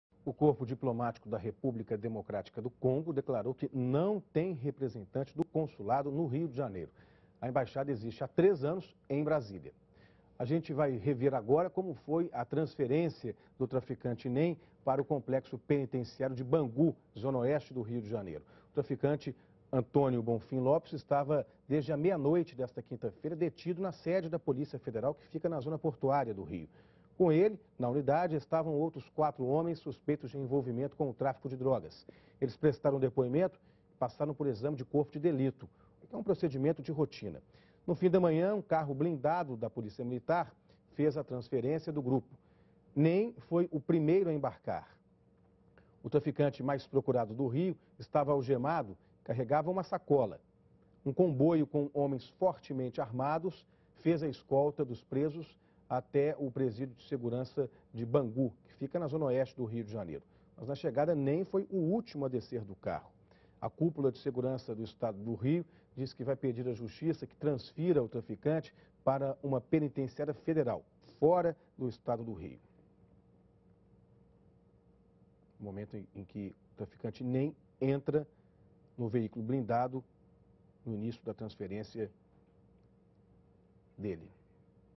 República do Congo nega elo com homem que tentou ajudar traficante Nem
O corpo diplomático da República Democrática do Congo declarou que não tem representante do consulado no Rio de Janeiro. O traficante Nem foi transferido para o presídio de Bangu junto com outros bandidos presos na ultima quarta-feira (09).